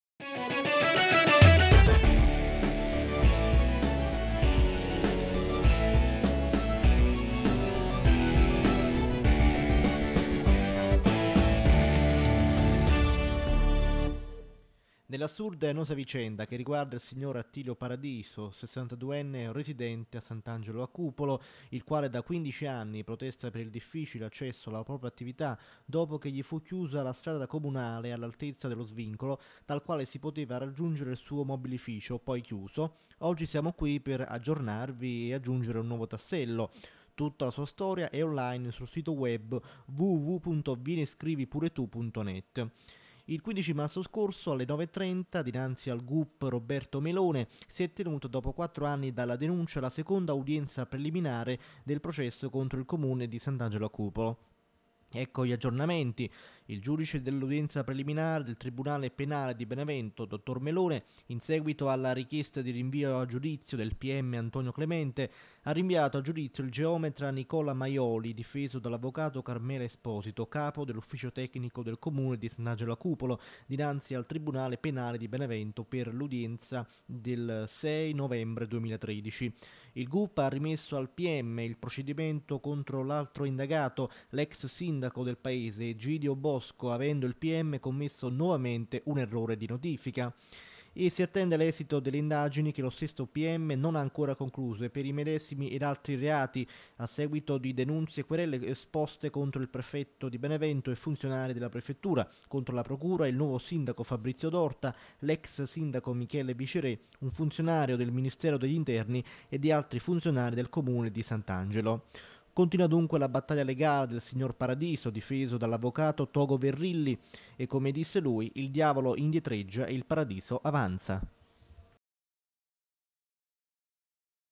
ELLE TV: Sant'Angelo a Cupolo - Vicenda Paradiso, c'è un rinvio a giudizio
Nell'assurda e annosa vicenda che riguarda il sig.Attilio Paradiso, 62enne  difeso dall'Avv. Togo Verrilli, aggiungiamo un nuovo tassello.  Il 15 marzo 2013, alle ore 9,30, dinanzi al GUP Roberto Melone, si è tenuta, dopo 4 anni dalla denuncia, la seconda udienza preliminare del  processo contro il Comune di Sant'Angelo a Cupolo.

Ecco di seguito gli aggiornamenti:

il Giudice dell'udienza preliminare del Tribunale Penale di Benevento, Dr. Melone, in seguito alla richiesta di rinvio a giudizio del P.M., Antonio Clemente, ha rinviato a giudizio  il Geom. Nicola Maioli, difeso dall'Avv. Carmela Esposito(capo dell'Ufficio Tecnico del Comune di S.Angelo a Cupolo) dinanzi al Tribunale Penale di Benevento per l'udienza del 06/11/2013.
il GUP ha rimesso al PM  il procedimento contro l'altro indagato, l'ex sindaco Egidio Bosco, avendo il PM commesso nuovamente un errore di notifica;
si attende l'esito delle indagini che lo stesso PM non ha ancora concluso, per i medesimi ed altri reati, a seguito di denunzie-querele sposte contro il Prefetto e funzionari della Prefettura, contro la Procura, il nuovo Sindaco Fabrizio D'Orta, l'ex sindaco Michele Vicerè, un funzionario del Ministero degli Interni ed altri funzionari del Comune di Sant'Angelo a Cupolo.
Continua dunque la battaglia legale del sig.Paradiso e  come disse lui: 'Il diavolo indietreggia ... e il Paradiso avanza'